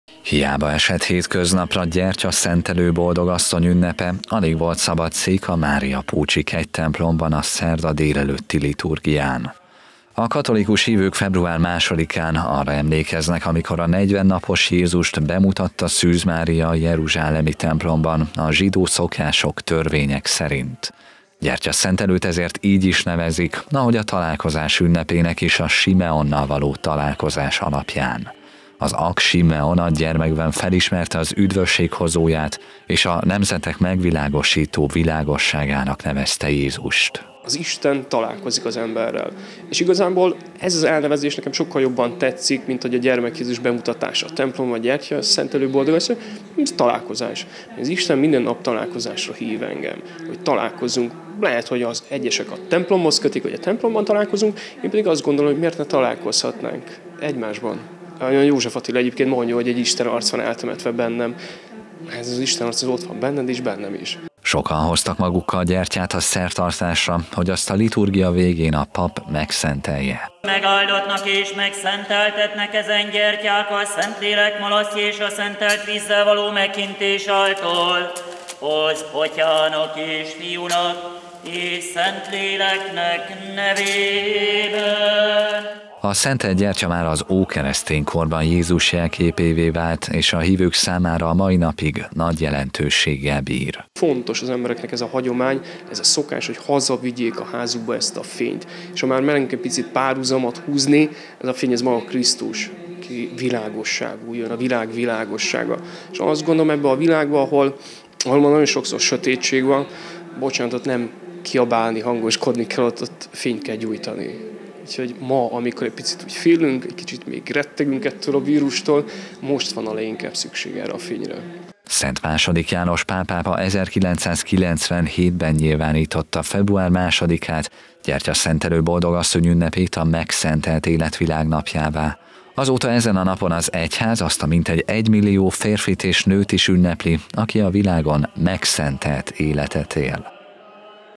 Gyertyaszentelő Boldogasszony
Gyertyaszentelő Boldogasszony napja van ma. Ekkor mutatta be a 40 napos Jézust anyja, Szűz Mária a jeruzsálemi templomban. A szentelt gyertya Jézus egyik legrégebbi jelképe. Erre a bibliai eseményre emlékeztek ma a máriapócsi kegytemplomban.